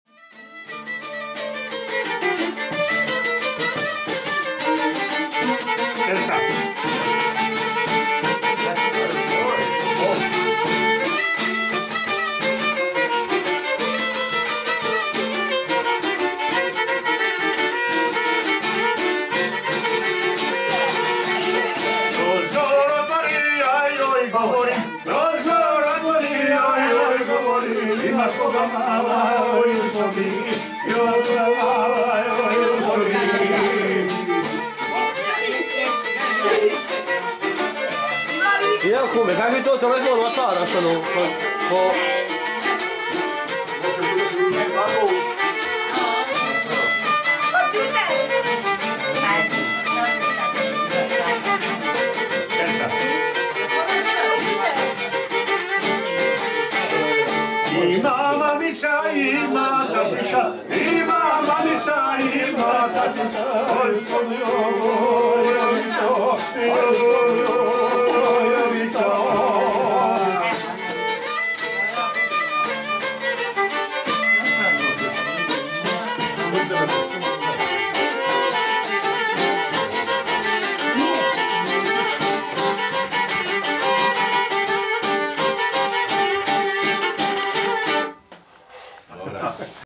VIOLINA JE GOVORILA U BABANOVIM RUKAMA
POKONI DRAGO BABIĆ ZVANI BABAN ZNAO JE RAZNE MUZIČKE IZVEDBE KAKO NA VIOLINI TAKO I NA ŠARGIJI BIO E VELIKI MUZIČKI UMJETNIK I JEDAN MEĐU NAJBOLIM